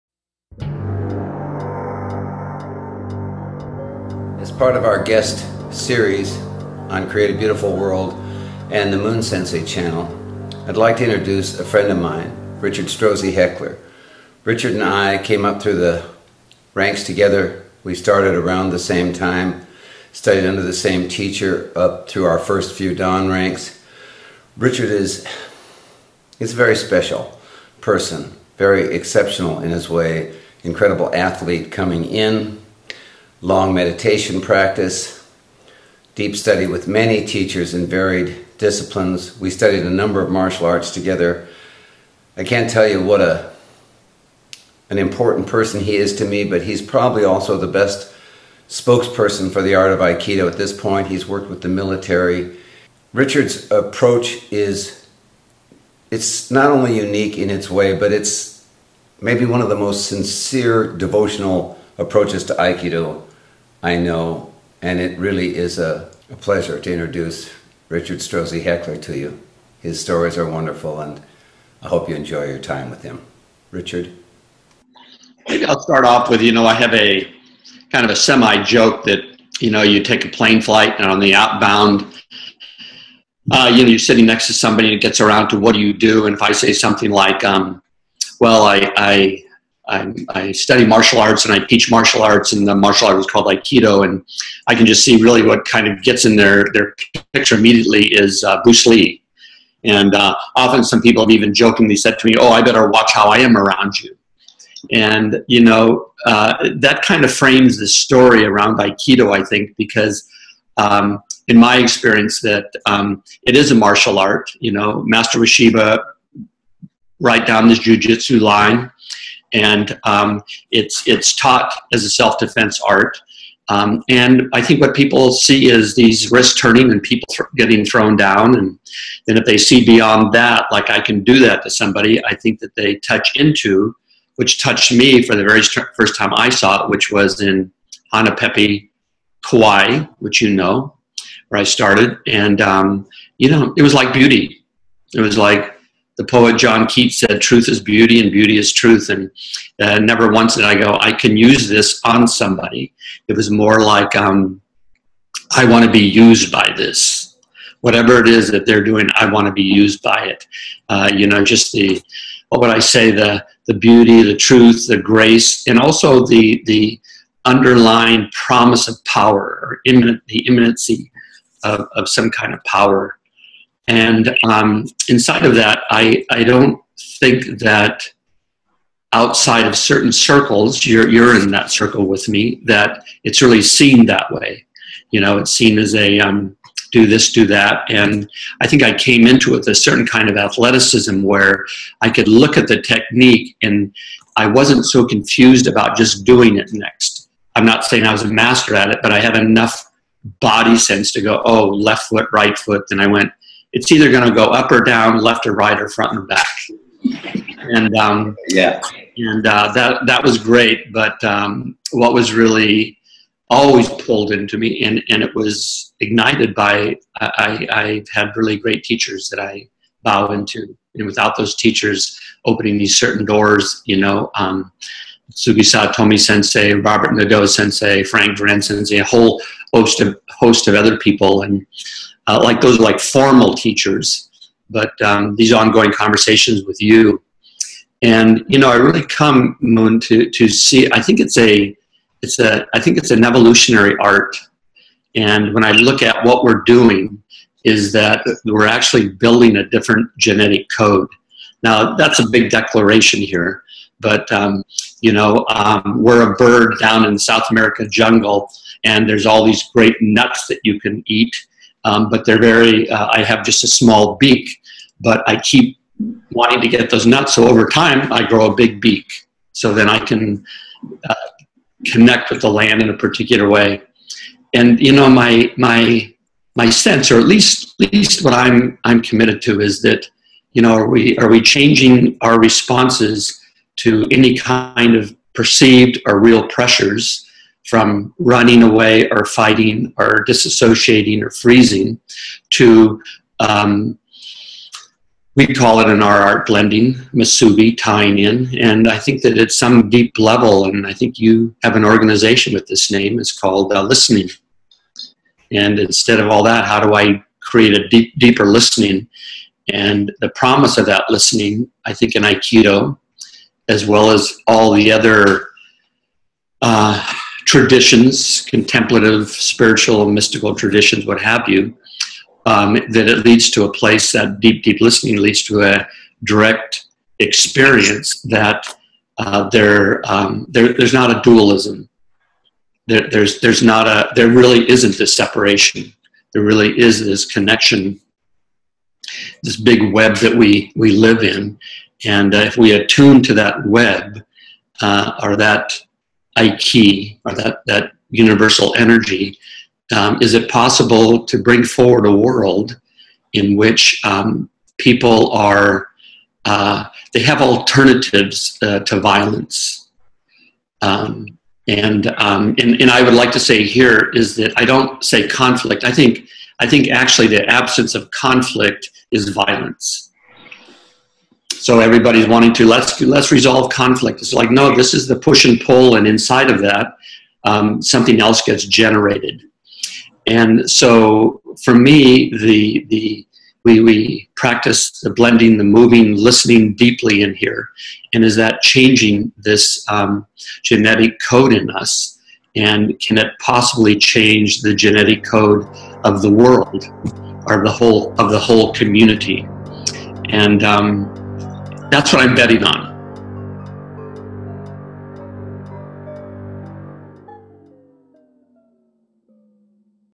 CABW invites Richard Strozzi Heckler: Part I
The focus of this series explores Aikido's contribution beyond any martial capability. And as much as i appreciate all the daily life applications, making better business people, better individuals, teachers, athletes etc.  I feel like the real gift he offered us is not seen enough.
"Rely on harmony
to activate your manifold powers,
pacify your environment
 and Create a Beautiful World."